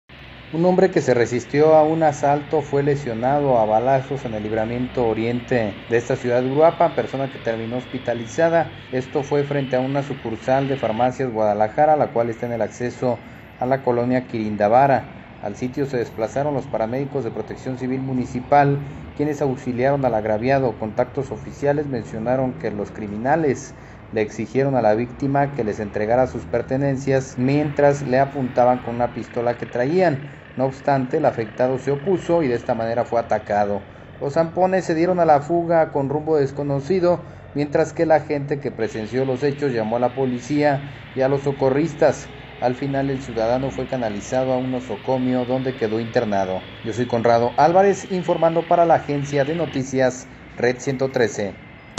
Peatón es baleado al oponerse a un robo
Uruapan, Mich.- Miércoles 17 de octubre de 2018.- Un hombre que se resistió a un asalto fue lesionado a balazos en el Libramiento Oriente de esta ciudad Uruapan, persona que terminó hospitalizada, dijeron fuentes policiacas.

El suceso fue este miércoles, frente a una sucursal de Farmacias Guadalajara, la cual está en el acceso a la colonia Quirindavara en esta urbe. Al sitio se desplazaron los paramédicos de Protección Civil Municipal, quienes auxiliaron al agraviado.

Contactos oficiales mencionaron que los criminales le exigieron a la víctima que les entregara sus pertenencias, mientras le apuntaban con la pistola que traían, no obstante el afectado se opuso y de esta manera fue atacado.

Los hampones se dieron a la fuga con rumbo desconocido, mientras que la gente que presenció los hechos llamó a la Policía y a los socorristas. Al final el ciudadano fue canalizado a un nosocomio de esta población, donde quedó internado.